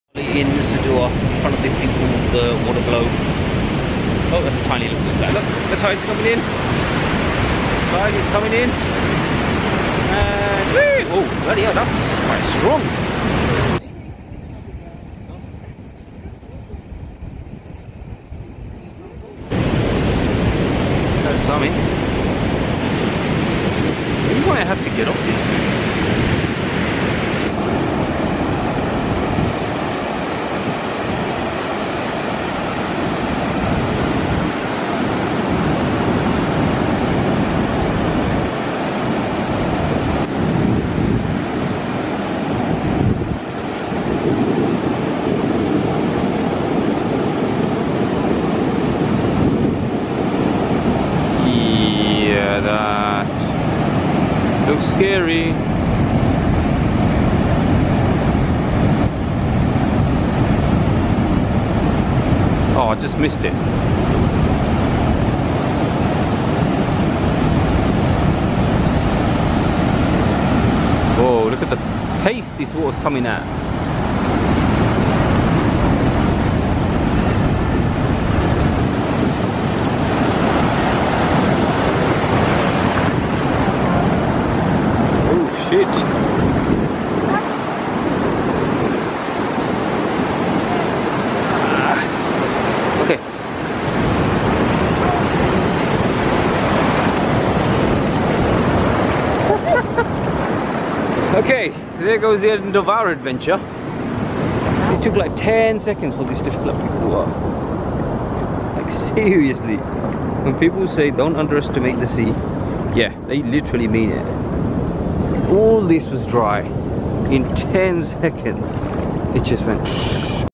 Waterblow Nusa Dua, Bali 2019
Check out this amazing waterblow. This is outside Grand Hyatt Hotel.